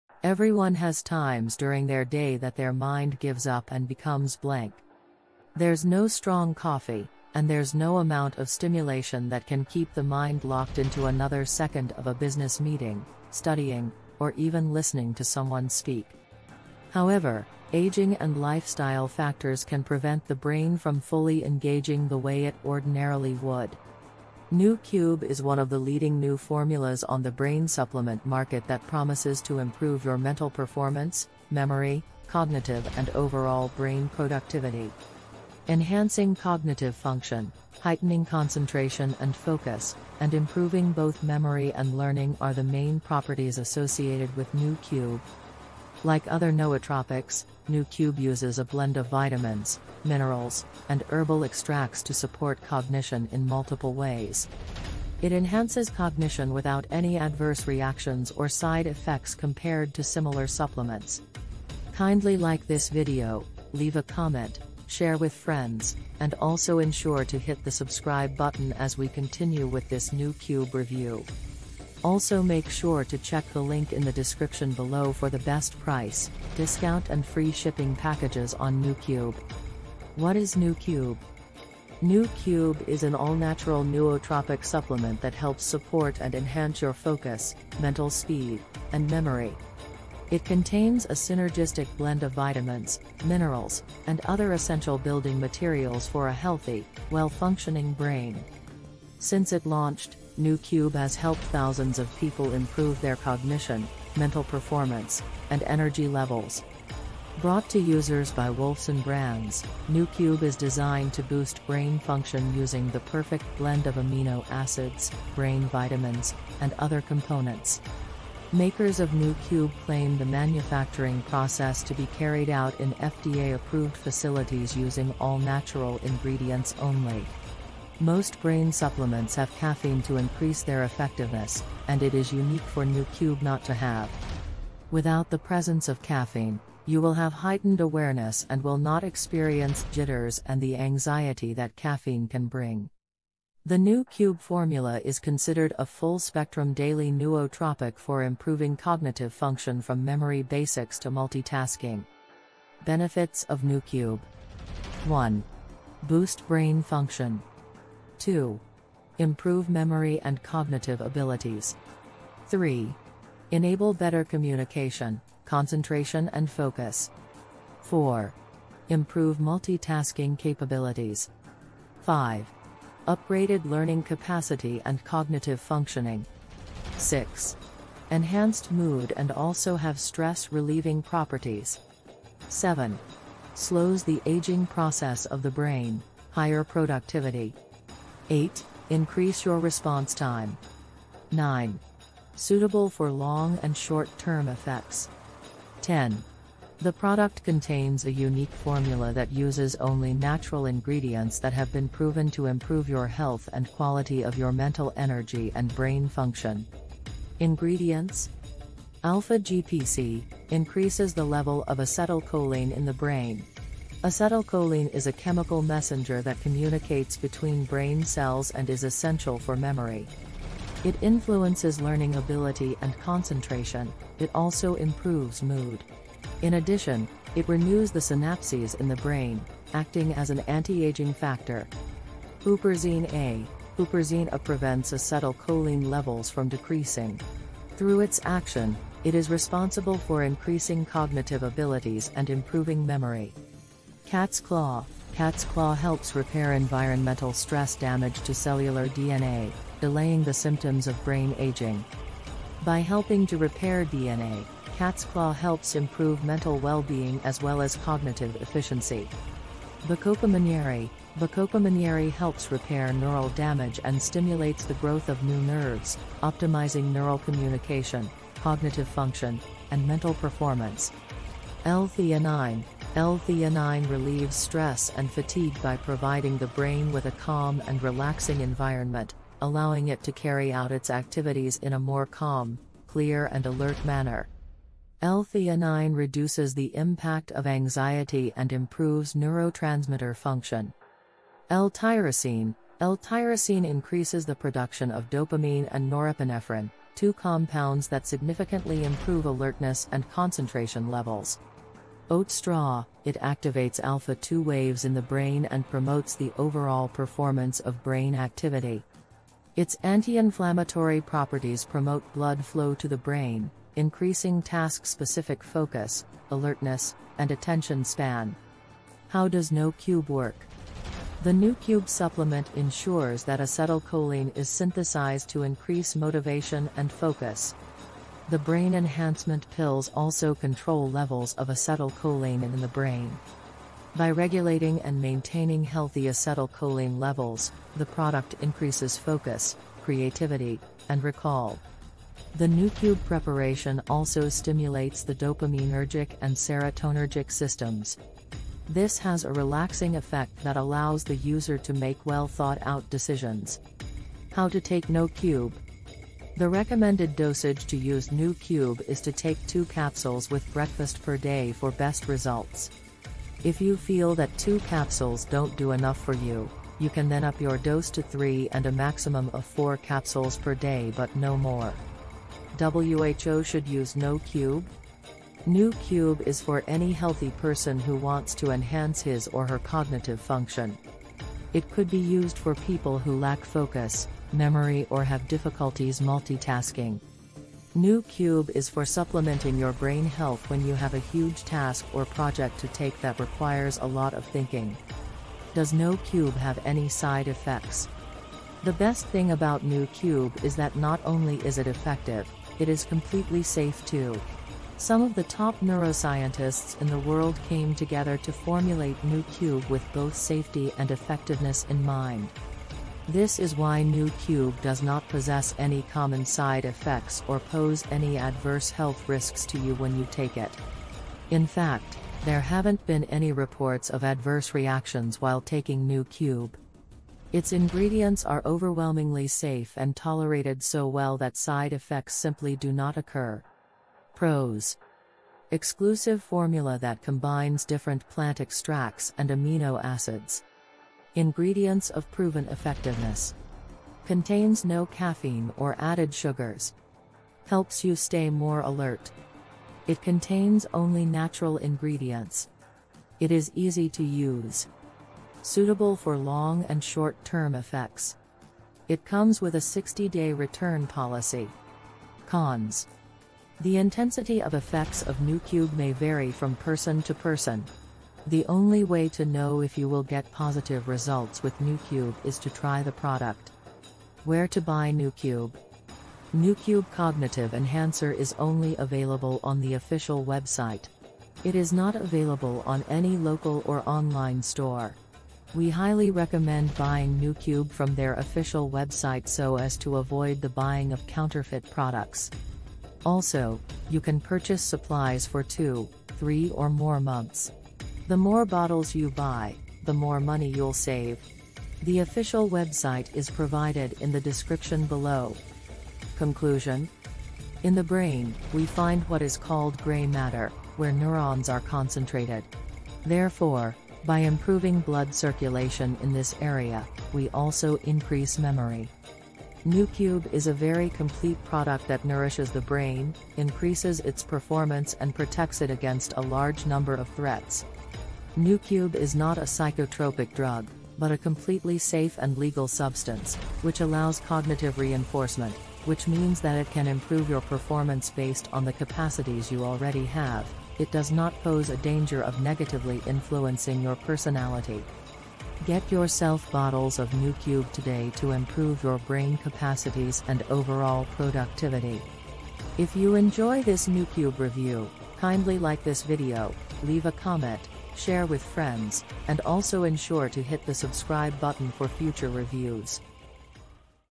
NOOCUBE REVIEW 2024: DOES IT REALLY IMPROVE BRAIN HEALTH? IMPORTANT THINGS YOU NEED TO KNOW
Noocube is a nootropic supplement, a class of chemical compounds that are thought to aid learning and memory while also helping to overcome natural and induced cognitive deficiencies. Noocube is a nutritional supplement that you can use on a regular basis to boost your cognitive performance.

Noocube contains a proprietary blend of natural, GMO-free ingredients such as Alpha GPC, Huperzine A, Cat’s Claw, Bacopa monnieri, oat straw, and amino acids L-theanine and L-tyrosine. Unlike many nootropics, Noocube does not contain caffeine.

Noocube can help maintain your brain health by lowering the negative impacts of free radicals, stress, and exhaustion when taken twice daily. Noocube's components have even been proved to help with Alzheimer's disease and dementia.

If you want to increase cognitive performance and improve mental speed, Noocube might be the brain supplement you’ve been waiting for.

Noocube aids learning and memory while also helping to overcome natural and induced cognitive deficiencies. Noocube is a nutritional supplement that you can use on a regular basis to boost your cognitive performance.

Best of all, some of the chemicals in NooCube are supposed to work within 30 to 45 minutes, so unlike many other nootropic compounds, you won't have to wait weeks to see results. NooCube's effects might last up to 10 hours, allowing you to perform when you need to the most.